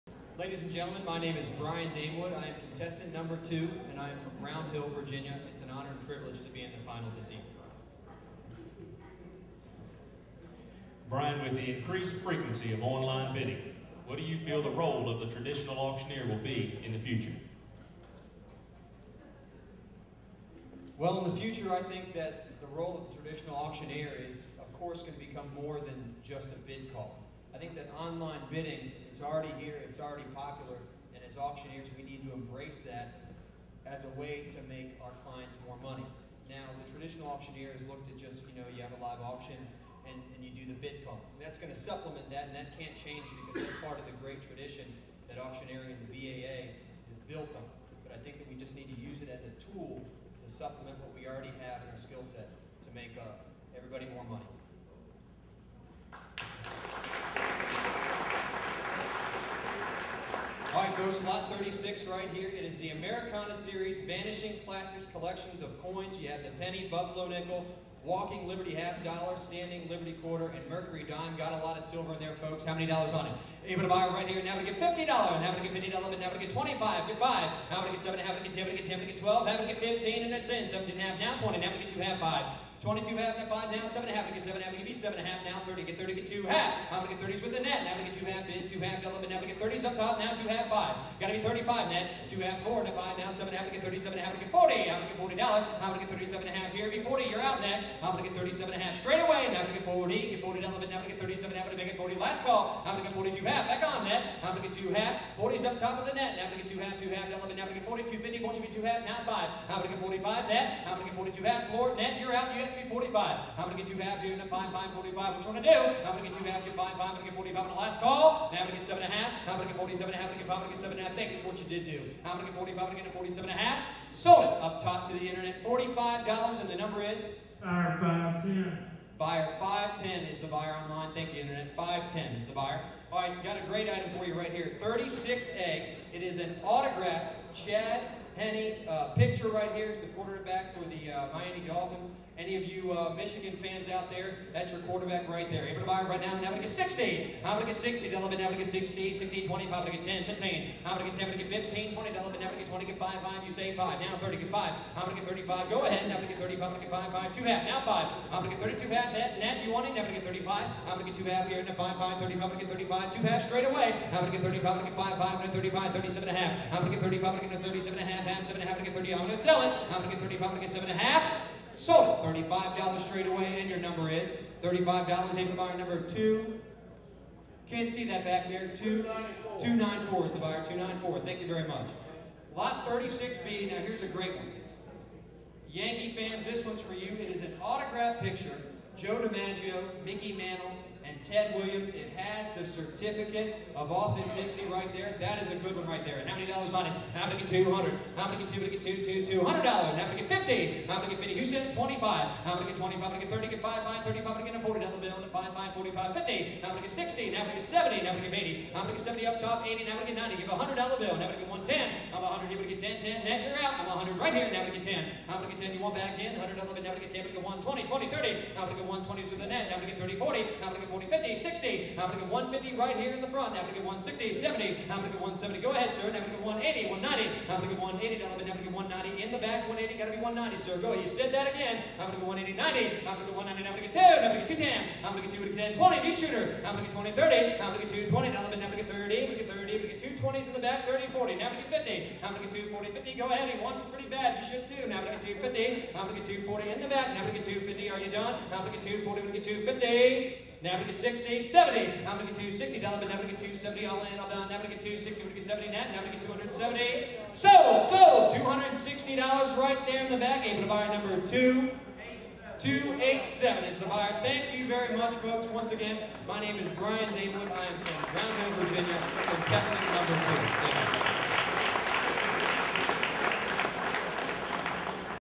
BrianDamewoodVirginiaAuctioneerChampionship.mov
Brian Damewood, Auctioneer from Leesburg, Virginia competing in the 2011 Virginia Auctioneers Association Champion Auctioneer Competition in Richmond.  Damewood finished 2nd.  He is an associate auctioneer with Tillett & Damewood, Auctioneers.